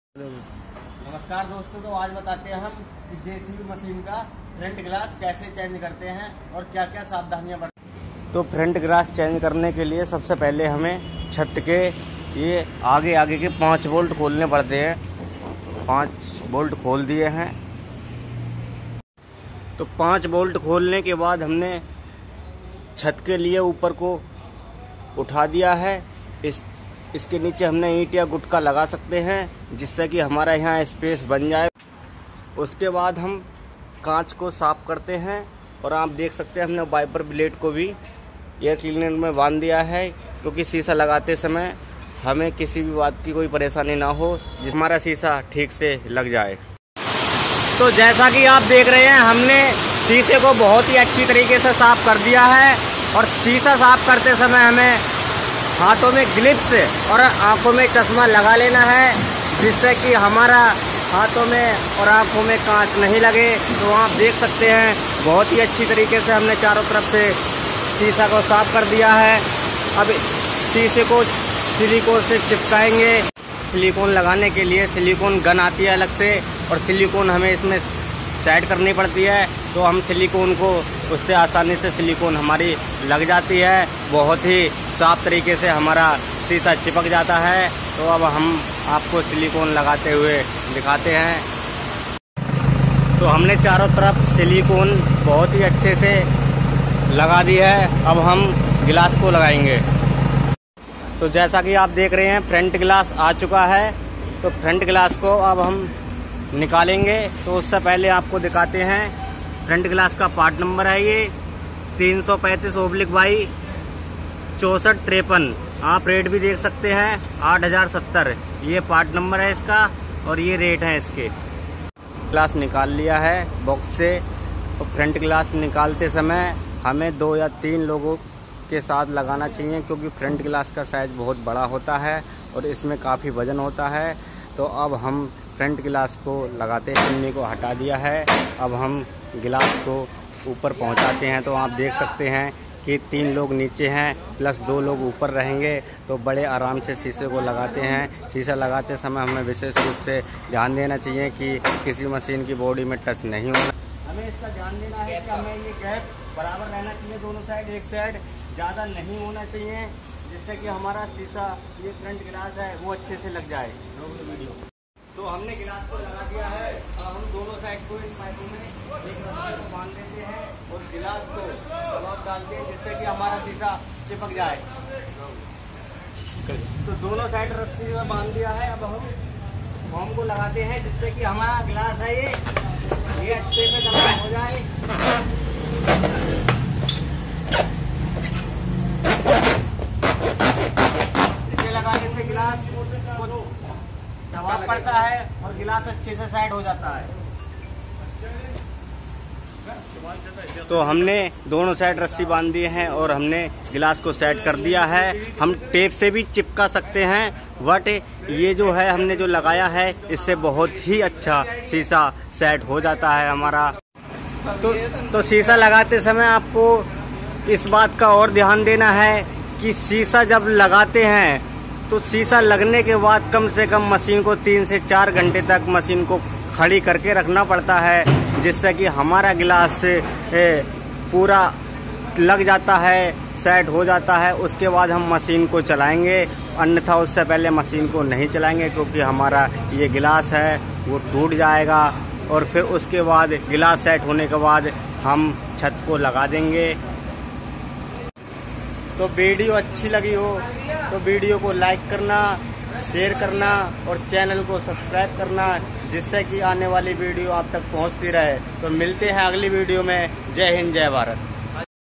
JCB मशीन का फ्रंट ग्लास कैसे चेंज करते हैं और ग्लास लगाते समय किन किन बातों का ध्यान रखना चाहिए
JCB मशीन का फ्रंट ग्लास कैसे चेंज करे और ग्लास लगाते समय किन किन बातों का ध्यान रखना चाहिए
jcb machine ka front glass kaise change kre
jcb machine ka front glass kaise change hota hai